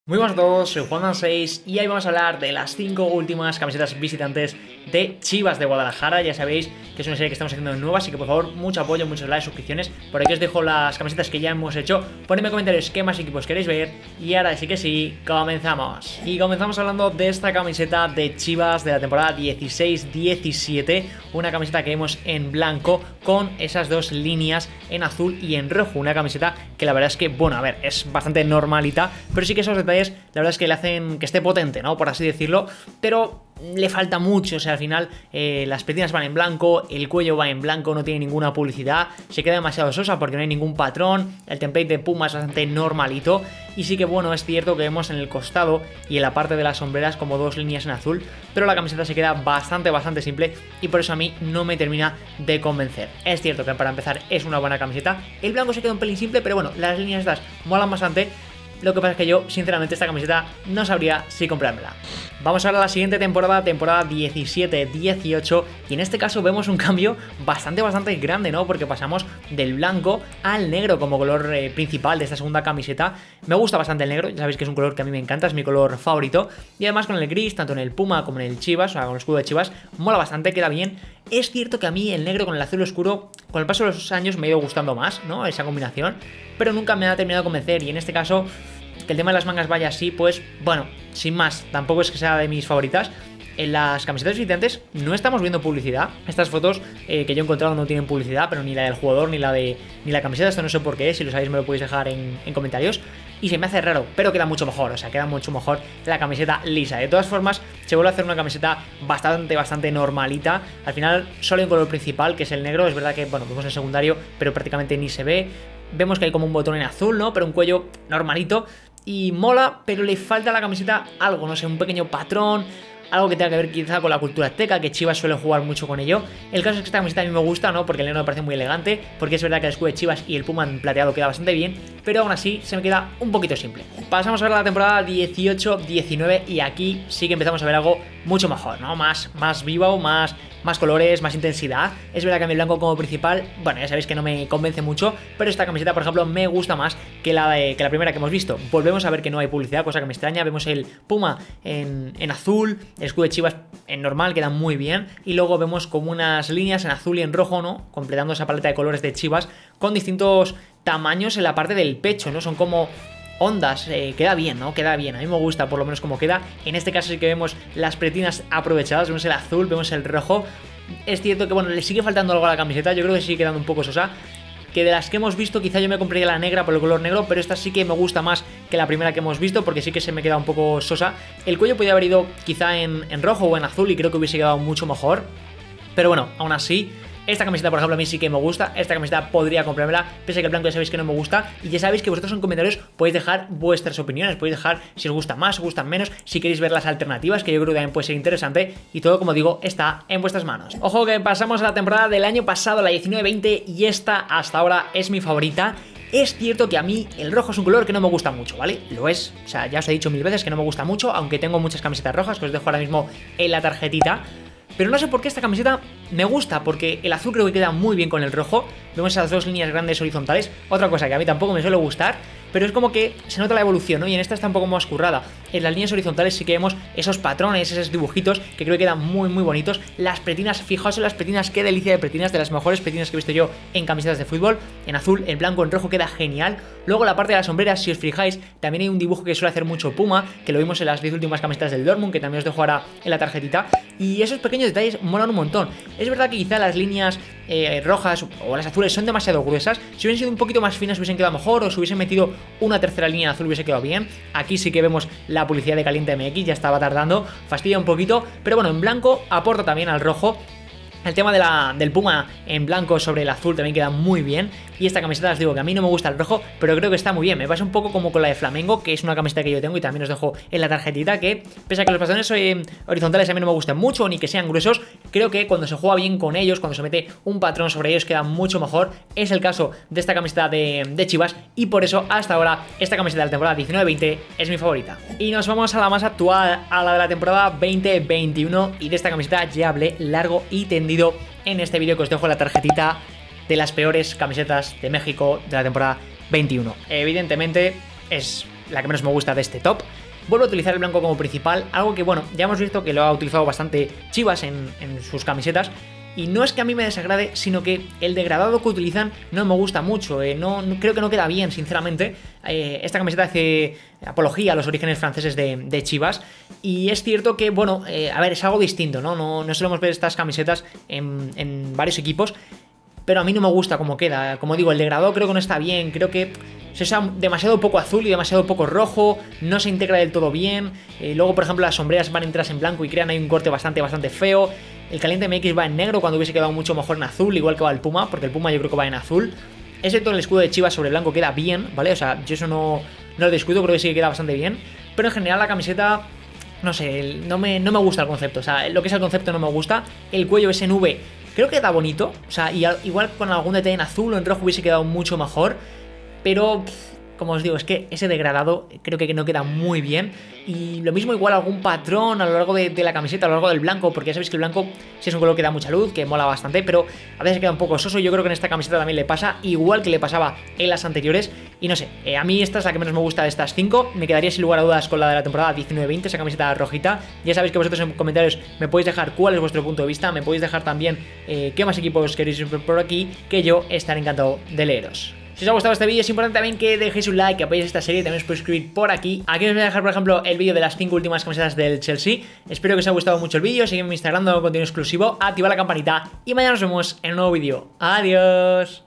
Reaccionando a las 5 ULTIMAS CAMISETAS VISITANTES de CHIVAS
Hoy reacciono a todas las camisetas visitantes de CHIVAS desde 2016